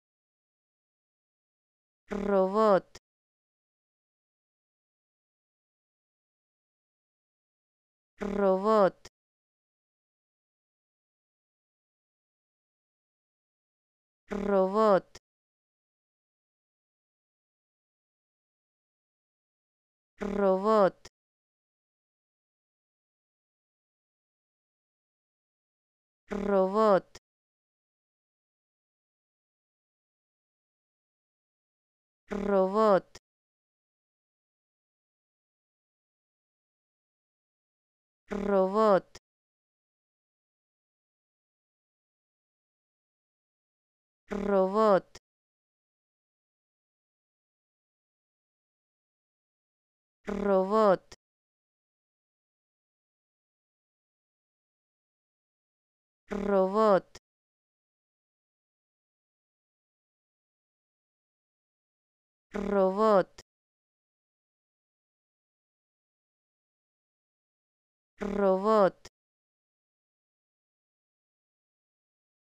Como Pronunciar ''Robot'' Correctamente en Español
Escucha la pronunciación de ''Robot'' y aprende a decirlo correctamente en Español.